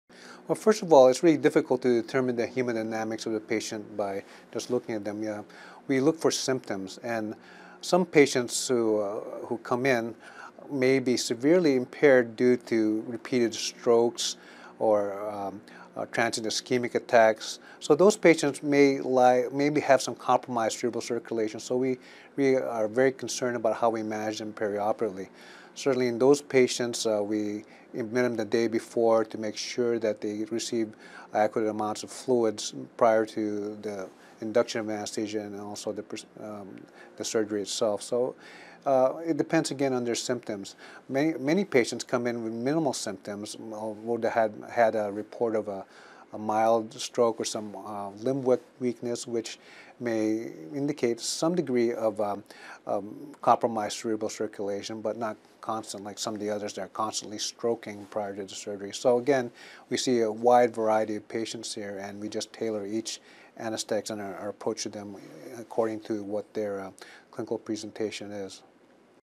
Intra-arterial Therapy for Strokes in Children
Darren B. Orbach, MD, PhD, Neurointerventional Radiologist at Boston Children's Hospital, talks about the role of cerebral angiogram and intra-arterial therapy in the diagnosis and treatment of strokes in children.